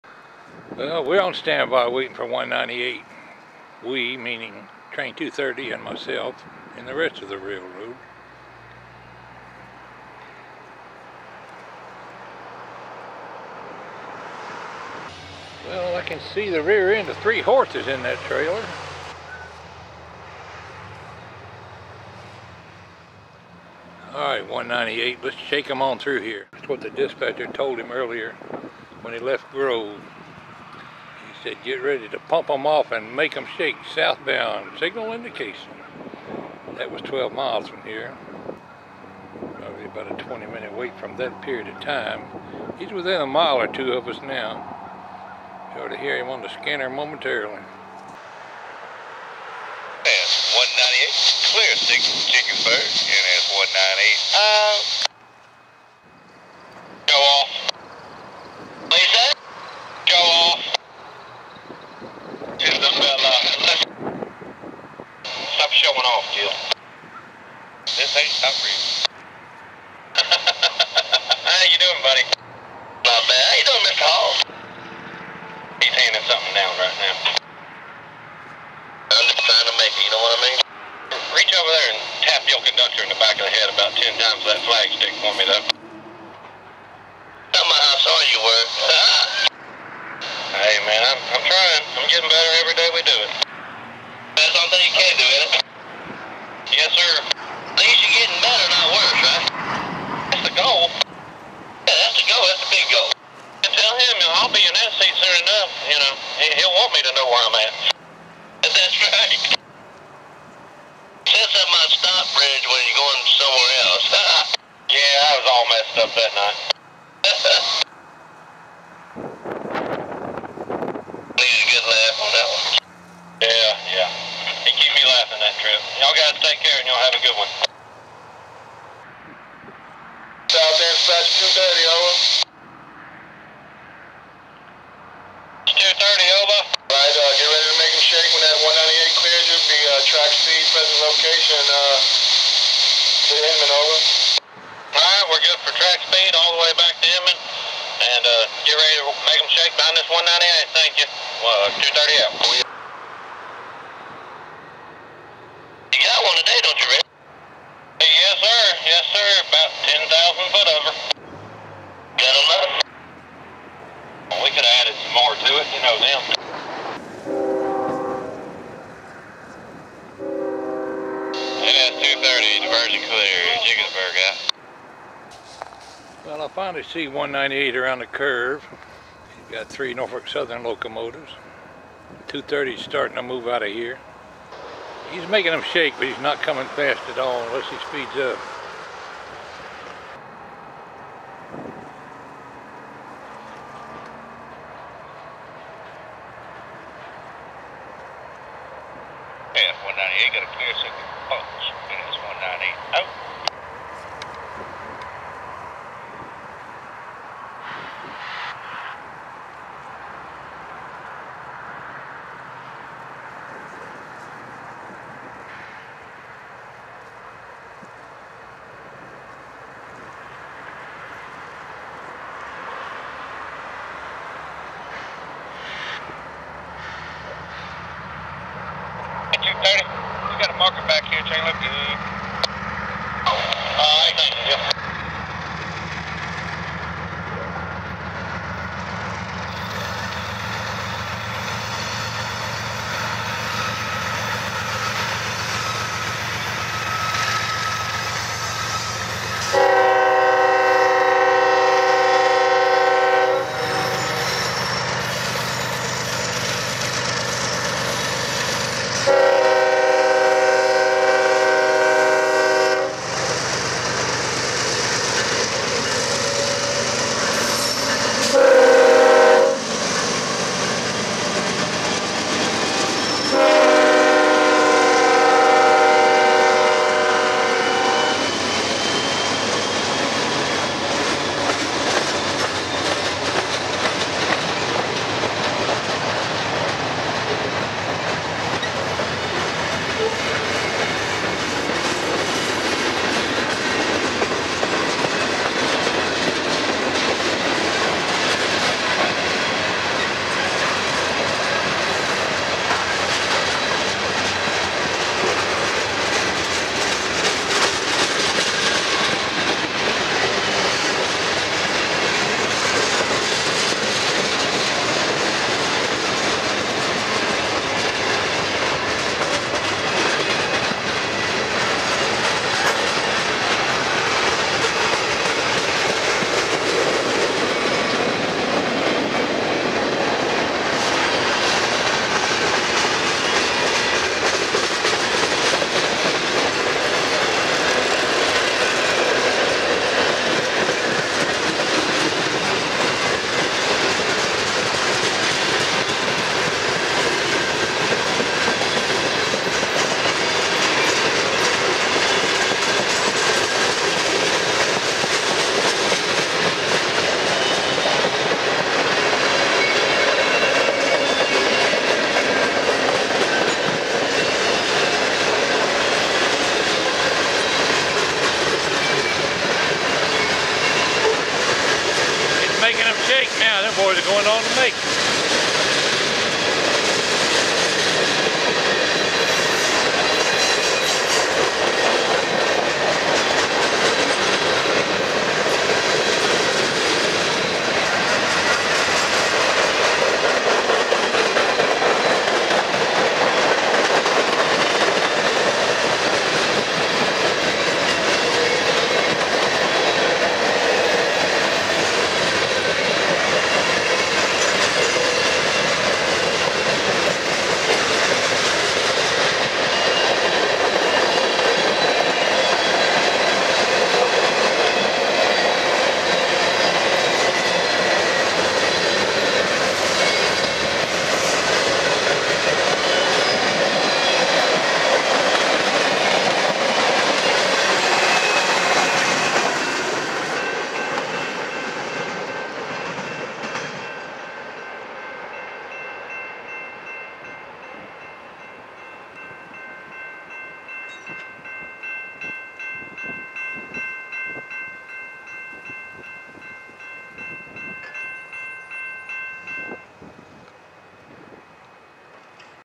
Make em shake Train 198 12-28-11.mpg
Video of NS Train 198, southbound by Bunch just south of Jenkinsburg Ga on 12-28-11.